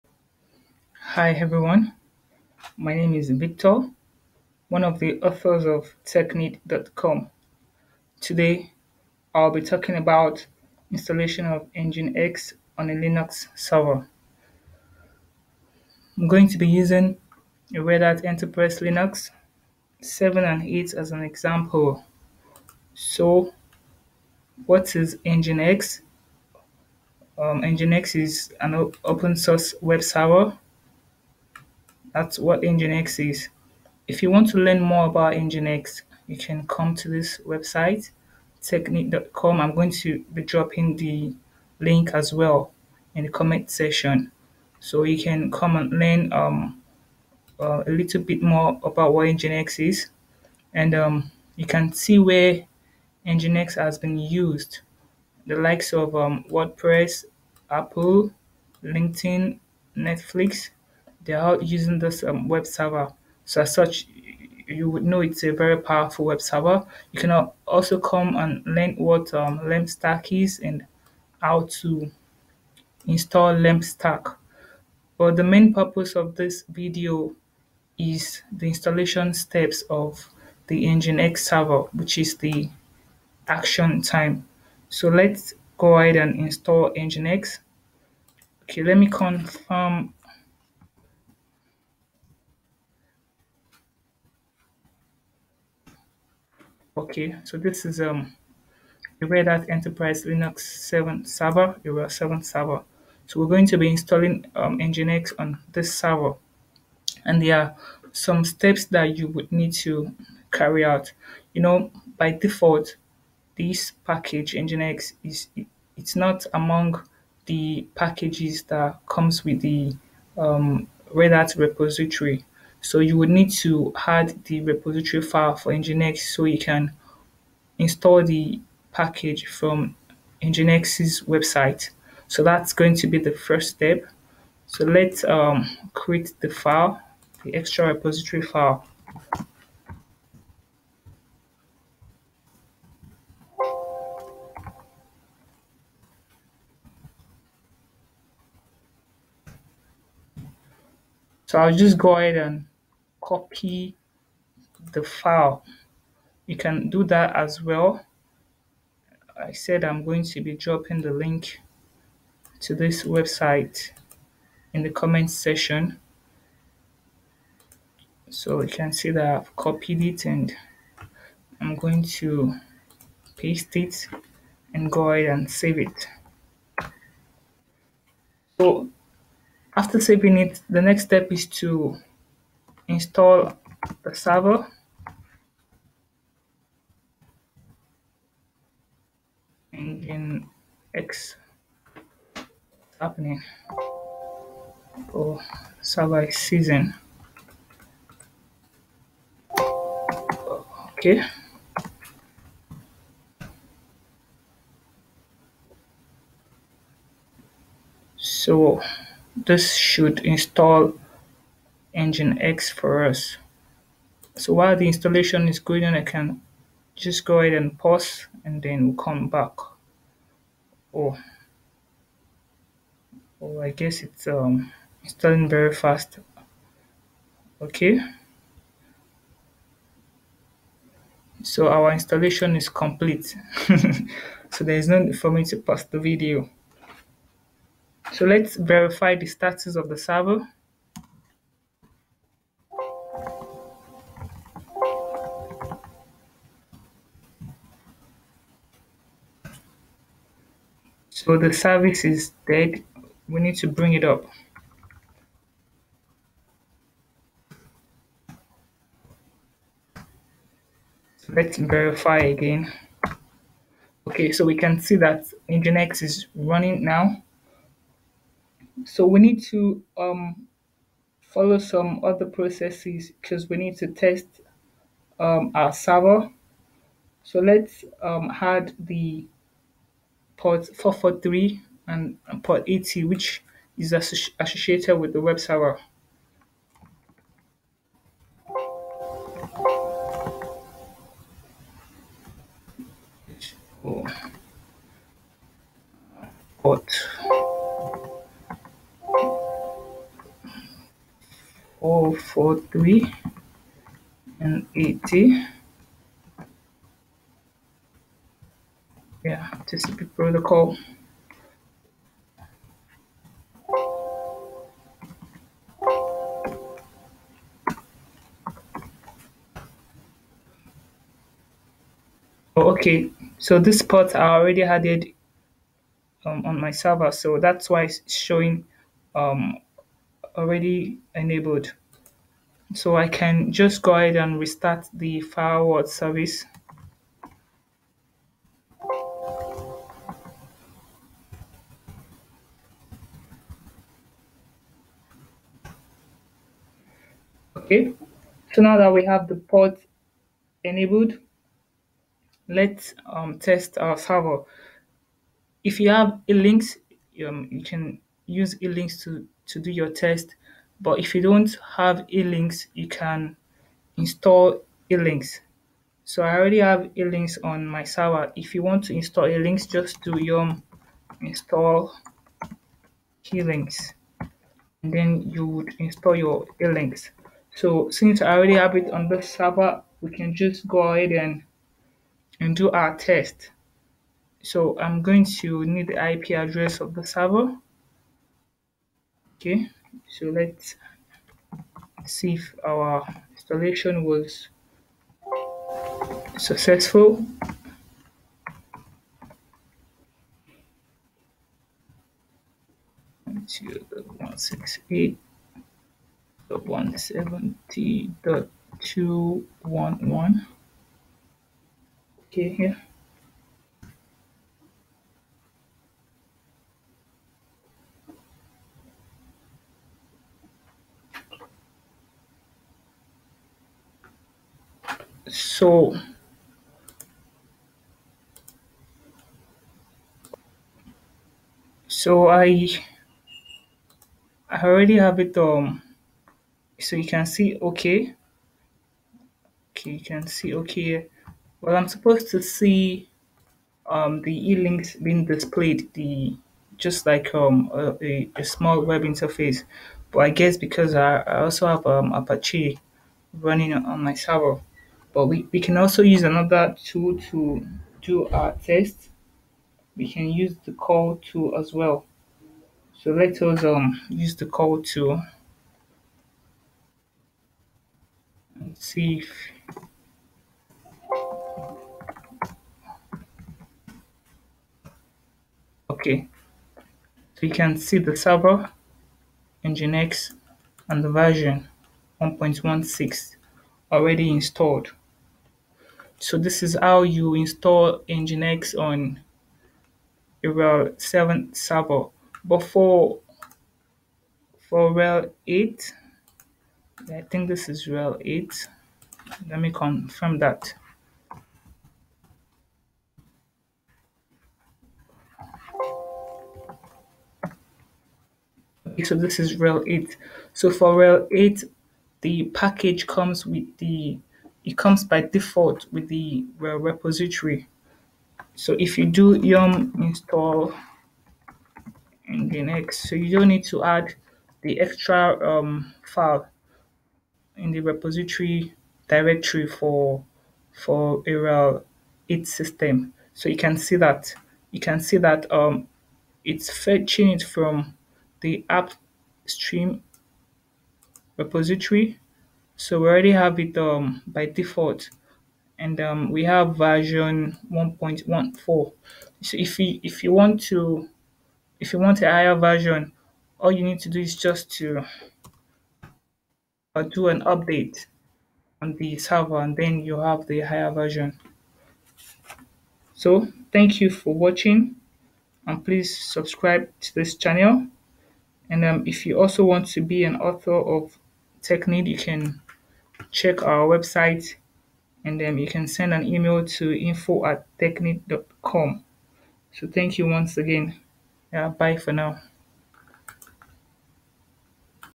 How To Install Nginx On Red Hat 7 & 8 Linux System
Learn the step by step process of how to install and test Nginx web server on a Red Hat Enterprise Linux server 7 & 8.